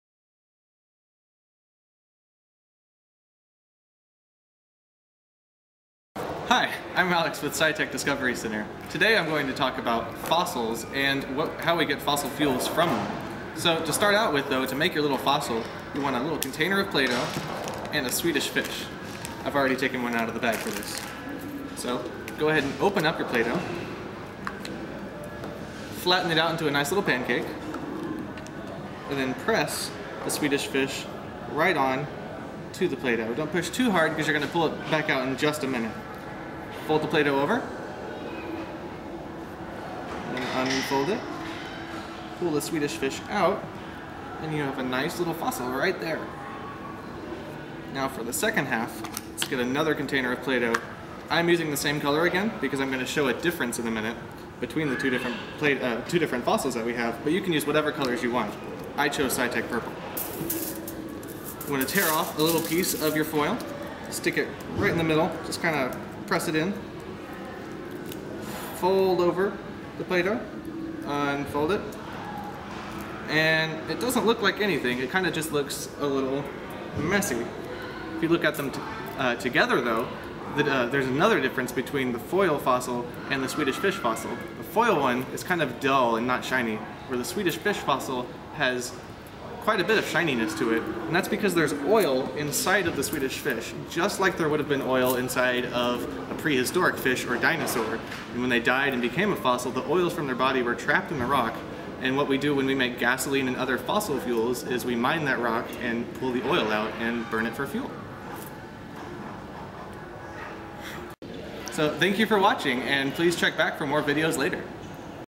Sci-Tech Labs: Fossil Fish
In this activity, we use a Swedish fish to model how fossils (and fossil fuel) is made.

Materials:
• 1 Swedish Fish
• 1 small container of playdough per item
• (Optional) Aluminum Foil or other items

Procedure
1. Take the playdough out of its packaging.
2. Make a pancake shape out of the playdough by using the palm of your hand to flatten the playdough.  Keep playdough's thickness equal throughout and make sure that it stays as thick as a Swedish fish.  The playdough represents the ground sediments.
3. Once you have your playdough pancake, take a Swedish fish from the bag.
a. Make this into a story by having this fish be a fish from long ago.  "He swam throughout the ocean, traveling far and wide.  He was an old but happy fish.  His time finally came and he passed away.  When he did, he fell to the bottom of the ocean.  After thousands of years, sediments covered him for hundreds of feet."
4. Fold the pancake in half so that the empty half folds over the fish.
a. "There was so much sand and dirt above him that his body was under a lot of pressure."
5. Place some pressure on top by using the palm of your hand.  Try not to squeeze so hard that you cannot open the playdough again.
a. "His body decomposed long ago so that not much was left."
6. Open up the playdough so that your fish and the marking shows.
7. Remove the fish so that now you have two markings on either side.
a. "By the time we found where the fish lay, this is what we found.  This is how we know the fish lived.  We can look at the markings to figure out how large the fish was, what type of fish was, and even what the fish liked to eat.  This is what it means to study fossils.  You try to figure out the life of the animal/plant, try to figure out how the world looked like, and finally, how it affected the future.  Now, sometimes different things happen.  Look at how glossy the fossil looks.   In the right conditions, the parts of the fish that were left behind can turn into a fuel.  In fact, that's where fossil fuels come from.  Many of the things we use (including electricity and plastic) are made from these fossil fuels.
8. (Optional) Use other materials and see what happens.  Like in the video, aluminum foil makes crevices but will not leave the glossy layer the Swedish fish makes.

Science:
• Fossil fuels are categorized as non-renewable resources.  Non-renewable means that if we use that resource, our planet cannot make more to resupply the amount we used.  It can also be used for something that takes a very long time (thousands of years) to make.
• Fossil fuels are made from past organisms and the chemical reactions that happened afterwards.  The energy these organisms had stored before their passing is the same energy we try to use when we burn them in engines or factories.
• There are multiple types of fossil fuels.  Each one is made by different biodegradable parts and has their own history on how they became a fuel source.  These fossil fuels are often found together in different depths.  Below is a list of the most widely used.
o Petroleum "crude oil" -- is made out of the biodegraded parts of animals.   Petroleum is a liquid fossil fuel used for a variety of different things including electricity generation, plastics, and medicine.  The finer version of petroleum is made from plankton and other microorganisms.  This version is what we use as gasoline for our cars and is also called oil.
o Coal -- is made out of the remains of terrestrial plants.  It is formed when the remains of these plants are put under pressure and hardened. Coal is a hard sedimentary rock and is the most abundant of the fossil fuels.  Just like with petroleum, different classes of coal also exist, each with their own energy outputs.    It is used mainly for the production of heat and electricity.  In the U.S., 50% of our electricity is derived from coal, usually with a steam turbine and generator.
o Natural Gas -- is a gas made out of the remains of microorganisms.  Mostly methane, natural gas is considered clean compared to the other two fossil fuels.  This means that there are low environmental impacts from burning natural gas.
• Use of fossil fuels has a variety of impacts on the environment. Greenhouse gas, acid rain and smog are three examples of the impact from their use.  There are also consequences when it comes to removal of these fossil fuels as forests and mountains have to be cleared to set up the equipment.  In recent news, fracking has become an issue as that can also pollute our water.  Lastly, transportation of the fuels has made environmental disasters in various areas of the world including the Gulf of Mexico and the waters of Alaska.
• Despite the problems associated, fossil fuels continue to be the most widely used fuel source due to their cheap economical nature.  While alternative renewable resources are being researched, all of them have their own pros and cons.  It is up to the people to find a balance.